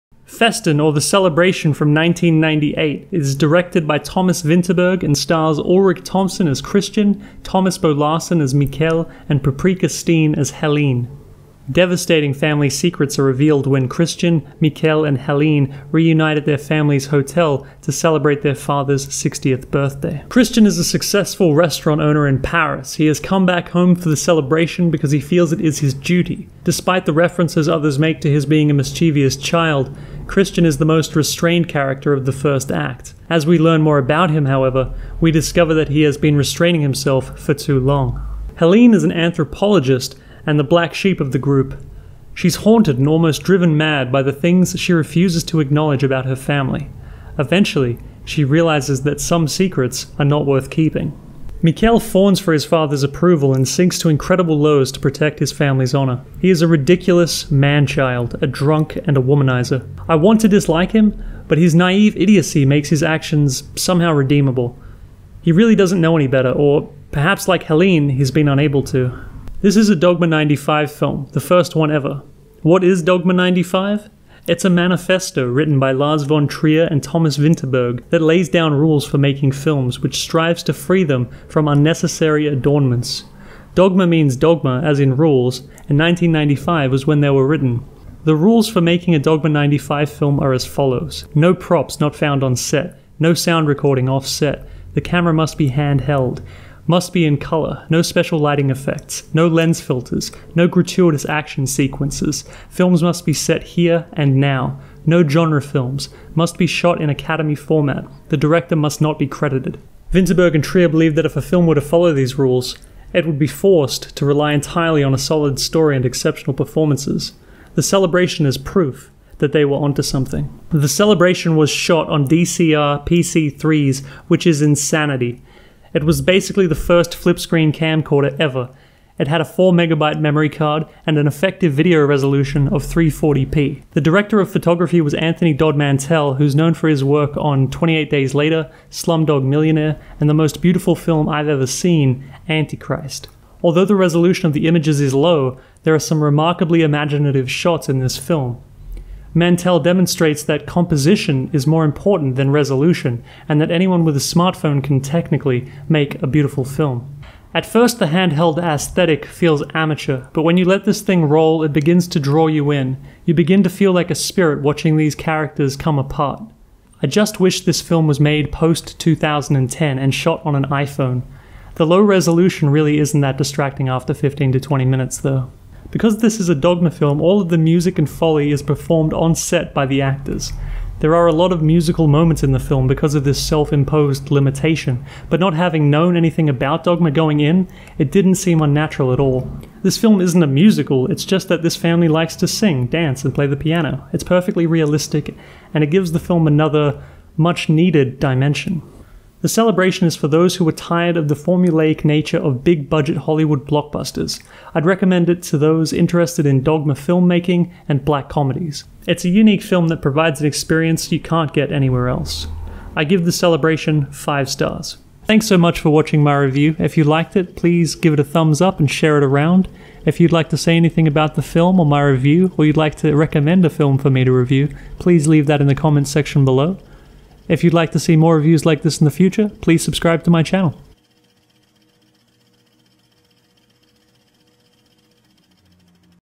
Festen The Celebration 1998 Film Review
A dark secret mars a Dane's (Henning Moritzen) 60th birthday celebration, attended by his family (Ulrich Thomsen, Thomas Bo Larsen) and friends.